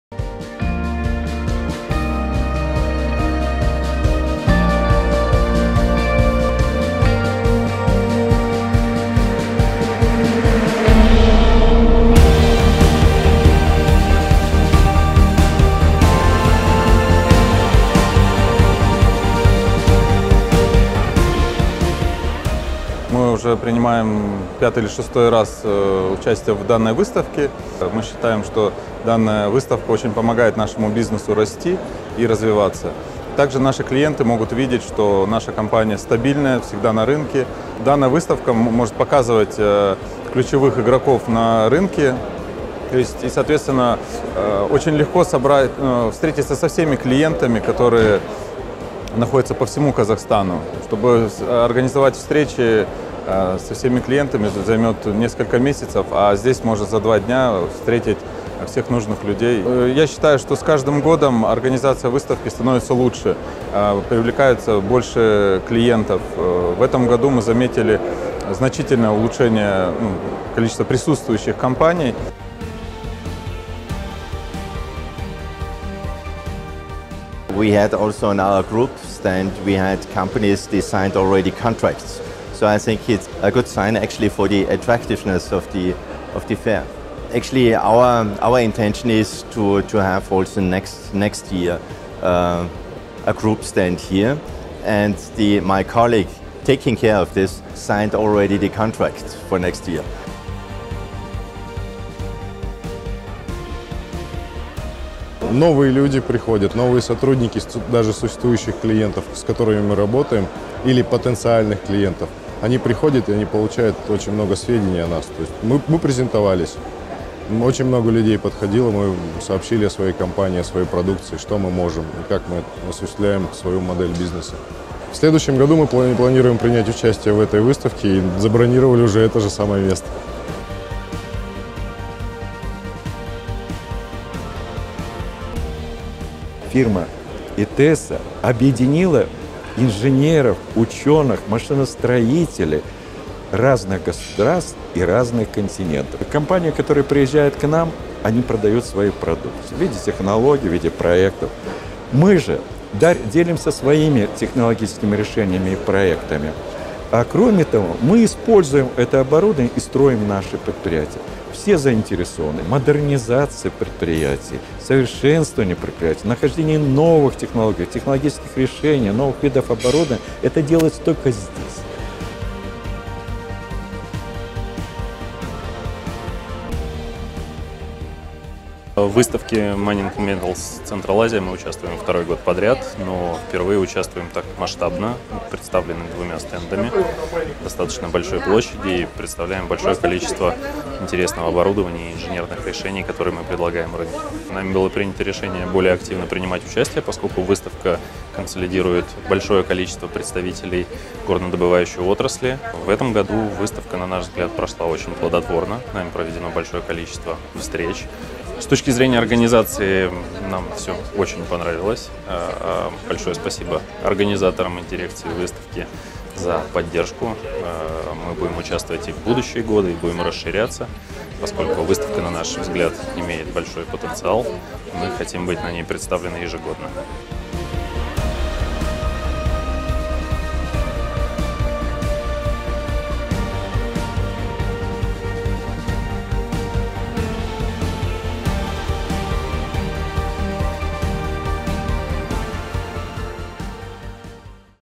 Mining and Metals Central Asia 2019 Exhibitor's Feedback
Read the reviews from the participants of the MINING AND METALLURGY CONGRESS ASTANA MINING & METALLURGY - what is unique and useful of the event and what opportunities the congress provideы for development.

The Congress traditionally took place at the Hilton Astana Hotel, Astana.

The Astana Mining & Metallurgy Congress is one of the largest industry events in the mining and metallurgy industry. AMM opens many platforms for dialogue and includes B2B and G2B meetings with the largest subsoil user companies and representatives of government agencies in Kazakhstan, an exhibition, networking and the Golden Hephaestus industry competition.

Since 2019, the organizers of the AMM Congress have been introducing new approaches to conducting events, changing the format from a presentation to a discussion platform. High-quality content of the program and a change in format allow us to fully reveal problematic issues in the industry and find ways to solve them.

We invite you to become part of this grand event, both as a participant and as a visitor.